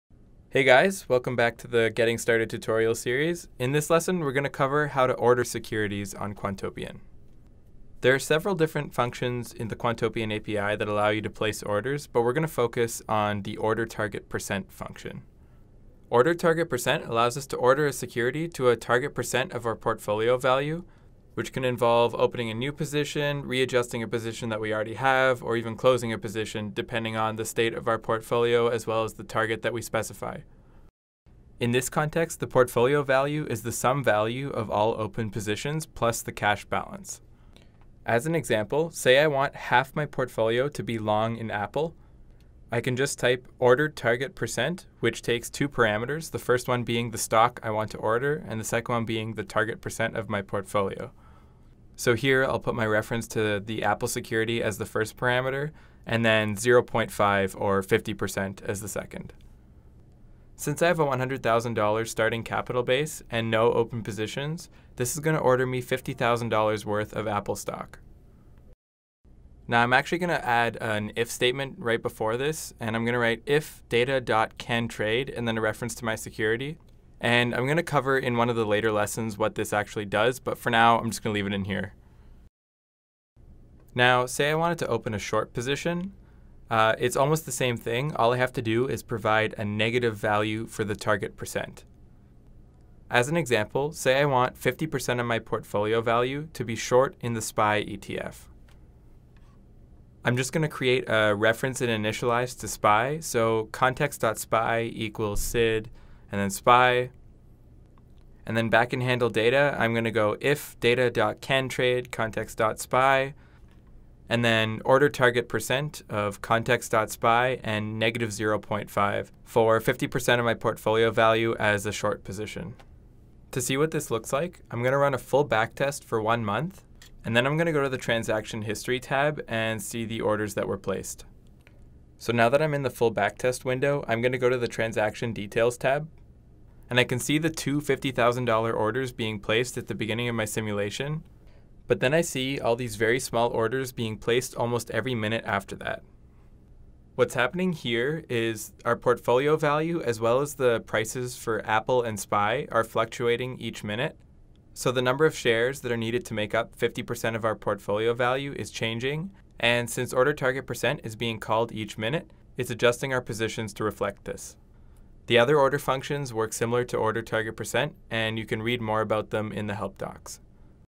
Getting Started Lesson 4: Ordering Securities
Learn how to place orders on Quantopian. In this video, we show how to place long and short market orders.